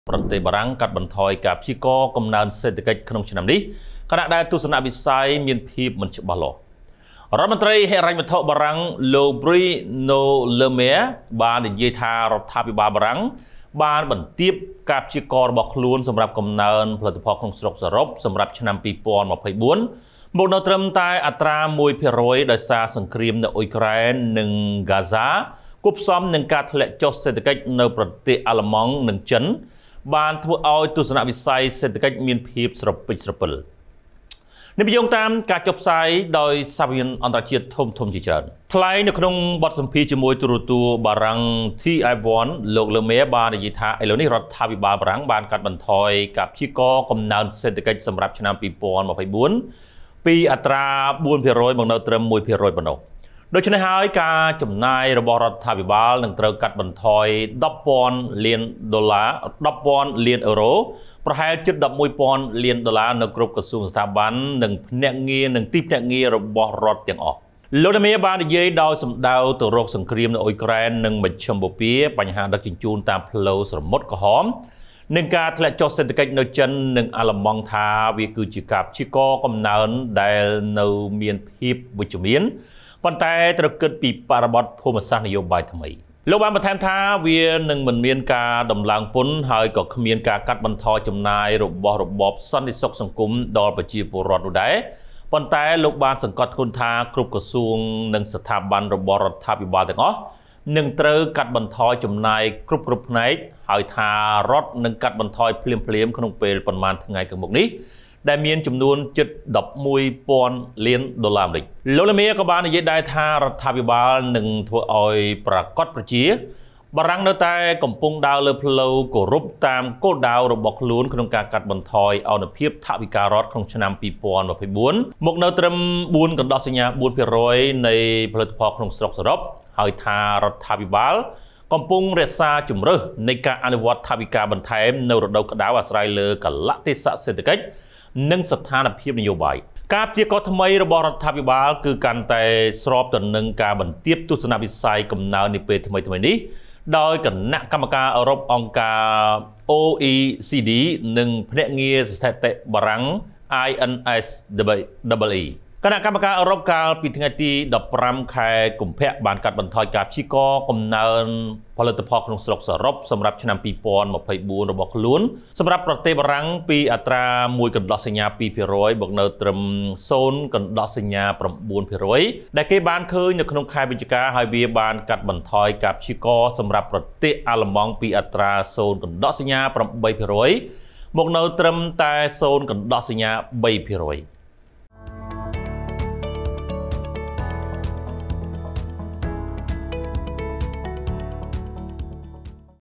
បារាំងកាត់បន្ថយព្យាករណ៍កំណើនសម្រាប់ឆ្នាំ២០២៤ខណៈដែលទស្សនវិស័យមានភាពមិនច្បាស់លាស់
ប្រទេសបារាំងកាត់បន្ថយព្យាករកំណើនសេដ្ឋកិច្ច ក្នុងឆ្នាំនេះ ខណៈដែលទស្សនវិស័យមានភាពមិនច្បាស់លាស់